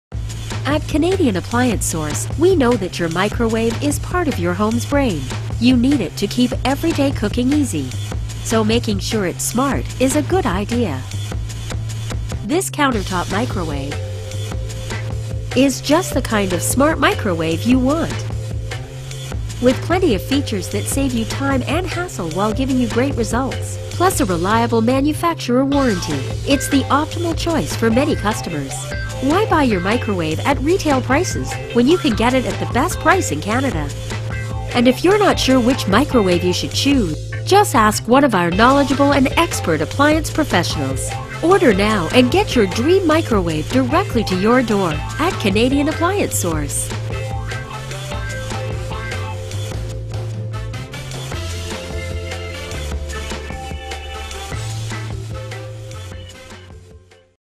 Panasonic NNCF781S Countertop Microwave
This Panasonic NNCF781S Countertop Microwave is brought you Panasonic NNCF781S Convection microwave, 1.0 cu.ft., 1400W, Flat glass door design, Inverter® turbo defrost, Auto reheat, Auto cook, Keep warm, Popcorn key, Quick minute, Child safety lock